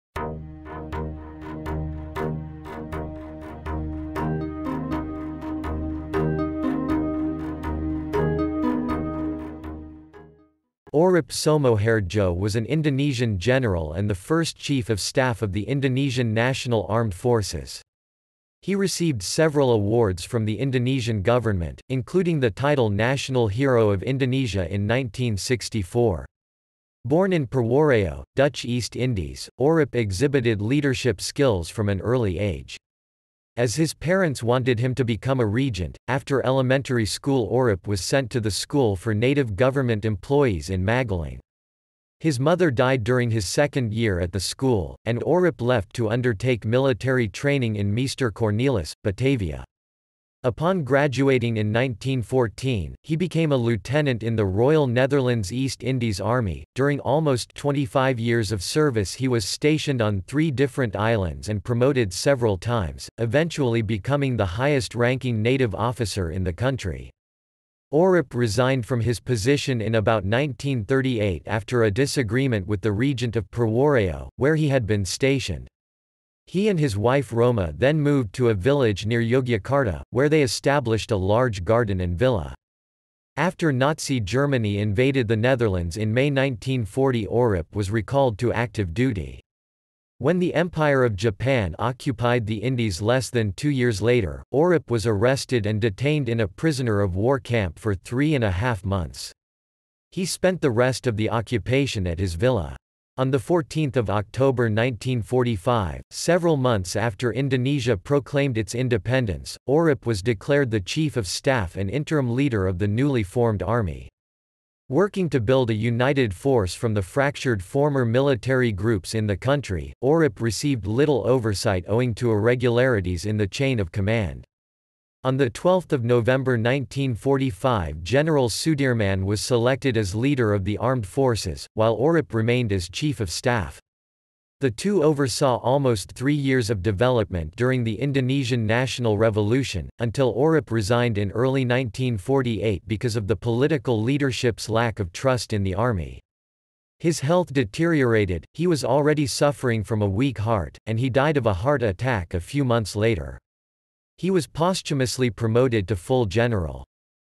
What is Oerip Soemohardjo?, Explain Oerip Soemohardjo, Define Oerip Soemohardjo
~~~ Oerip Soemohardjo ~~~

Title: What is Oerip Soemohardjo?, Explain Oerip Soemohardjo, Define Oerip Soemohardjo

Description: Oerip Soemohardjo was an Indonesian general and the first chief of staff of the Indonesian National Armed Forces. He received several awards from the Indonesian government, including the title National Hero of Indonesia in 1964. Born in Purworejo, Dutch East Indies, Oerip exhibited leadership skills from an early age. As his parents wanted him to become a regent, after elementary school Oerip was sent to the School for Native Government Employees in Magelang. His mother died during his second year at the school, and Oerip left to undertake military training in Meester Cornelis, Batavia . Upon graduating in 1914, he became a lieutenant in the Royal Netherlands East Indies Army; during almost 25 years of service he was stationed on three different islands and promoted several times, eventually becoming the highest-ranking Native officer in the country. Oerip resigned from his position in about 1938 after a disagreement with the regent of Purworejo, where he had been stationed. He and his wife Rohmah then moved to a village near Yogyakarta, where they established a large garden and villa. After Nazi Germany invaded the Netherlands in May 1940 Oerip was recalled to active duty. When the Empire of Japan occupied the Indies less than two years later, Oerip was arrested and detained in a prisoner-of-war camp for three and a half months. He spent the rest of the occupation at his villa. On 14 October 1945, several months after Indonesia proclaimed its independence, Oerip was declared the chief of staff and interim leader of the newly formed army. Working to build a united force from the fractured former military groups in the country, Oerip received little oversight owing to irregularities in the chain of command. On 12 November 1945 General Sudirman was selected as leader of the armed forces, while Oerip remained as chief of staff. The two oversaw almost three years of development during the Indonesian National Revolution, until Oerip resigned in early 1948 because of the political leadership's lack of trust in the army. His health deteriorated; he was already suffering from a weak heart, and he died of a heart attack a few months later. He was posthumously promoted to full general.